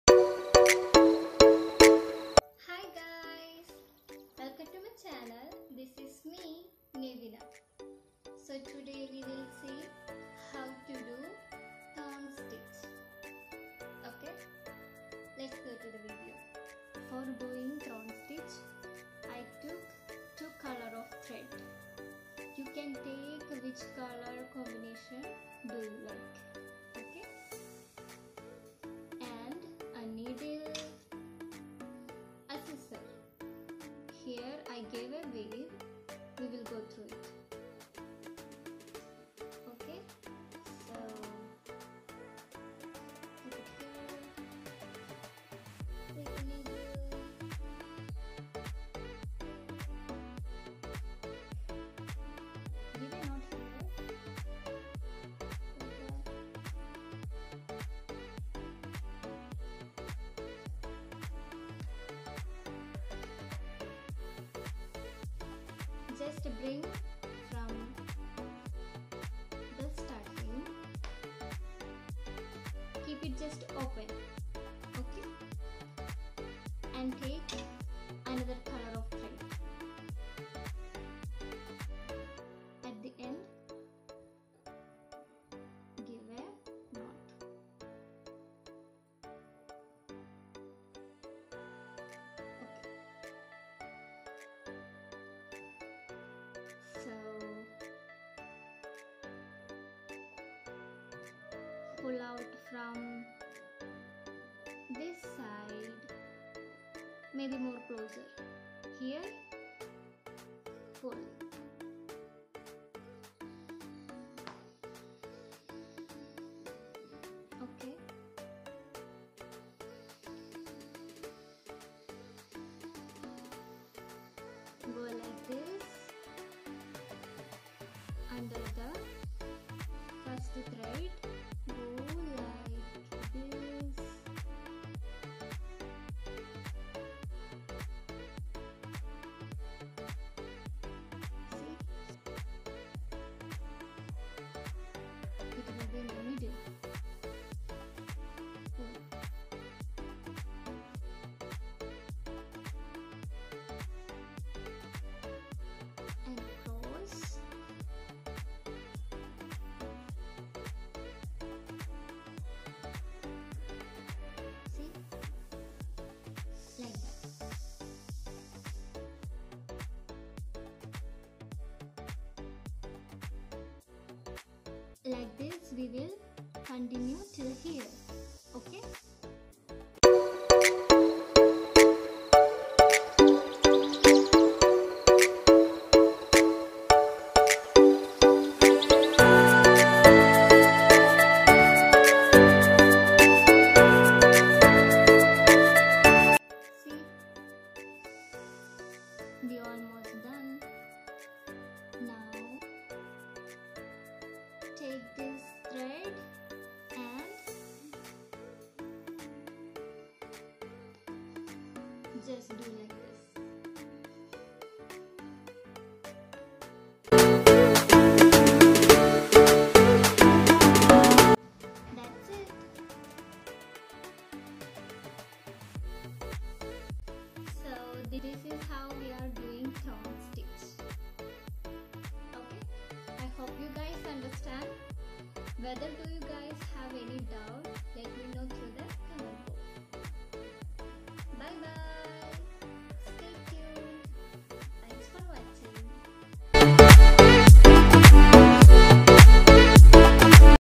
How to do Thorn Stitch embroidery by Nevina
Embroider with nevinasfashionworld

✴️ About Thorn Stitch
The Thorn Stitch is a variation that shows how the stitches can be stitched in different patterns to anchor down the laid thread. This stitch uses half Herringbone Stitches to pin down the thread to the fabric giving it an attractive look. The structure looks like a stem of thorns, explaining the name.